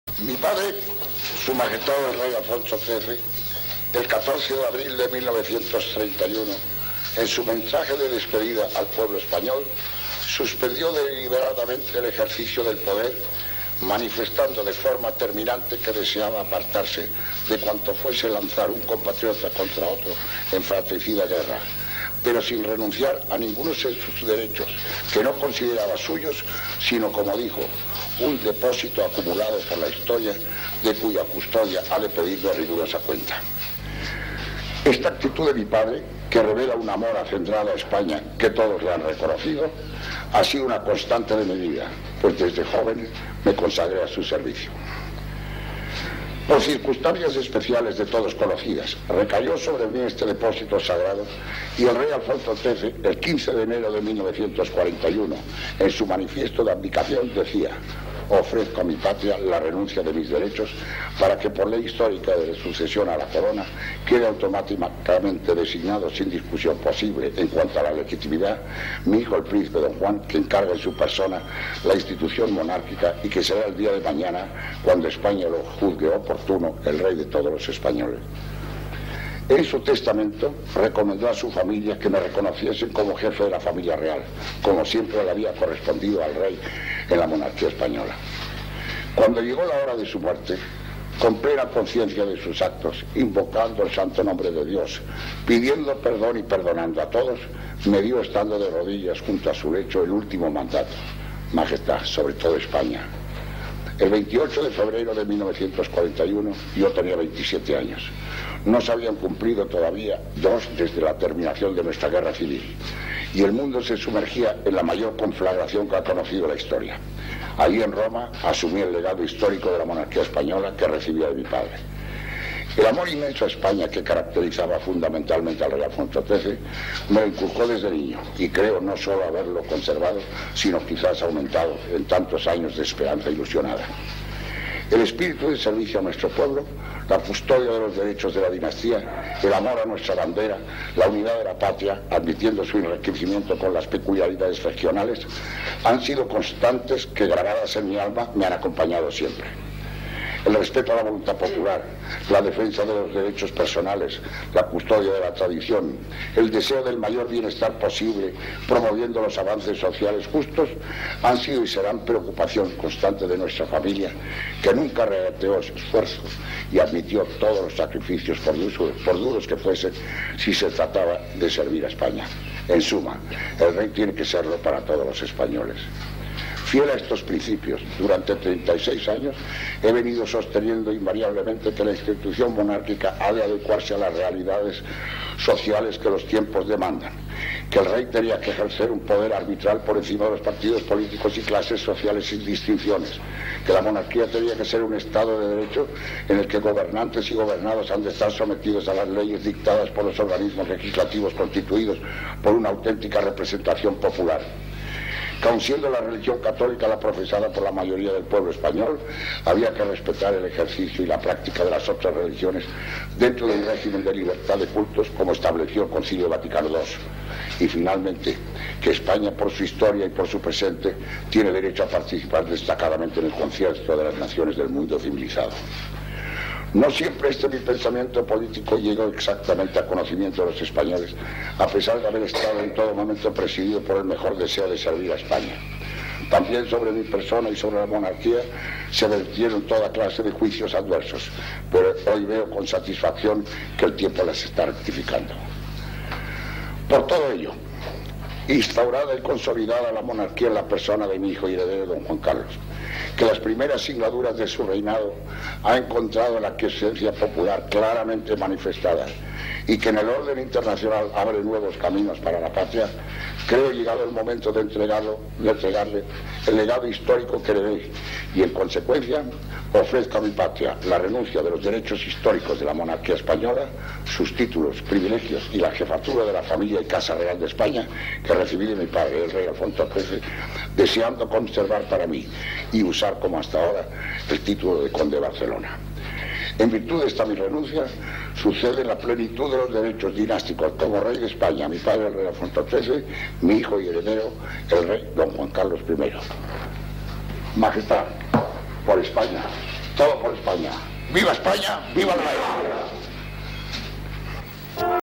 RENUNCIA DE DON JUAN DISCURSO COMPLETO.mpg
El 14 de mayo de 1977, Don Juan de Borbón renunció oficialmente a su derecho sucesorio, en favor de su hijo Juan Carlos.